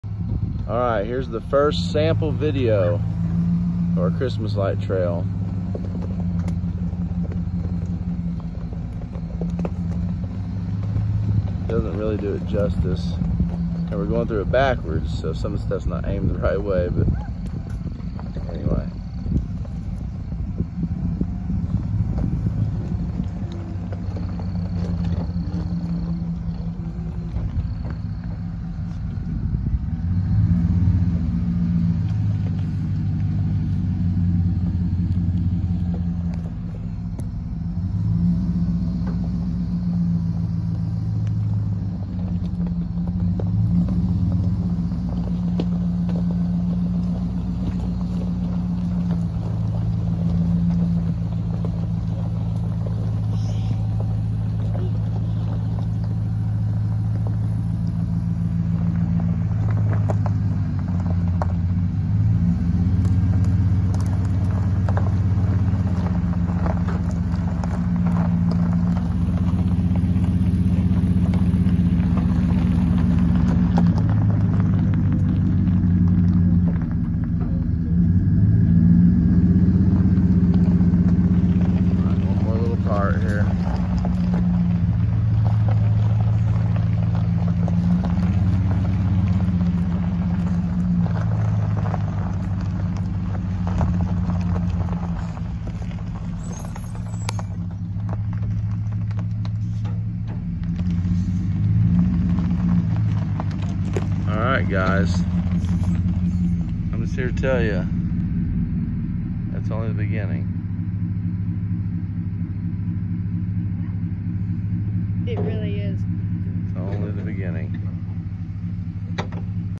Christmas Light Trail Preview 2021
This was the preview video￼ for our 2021 Christmas Light Trail. The first year doing it.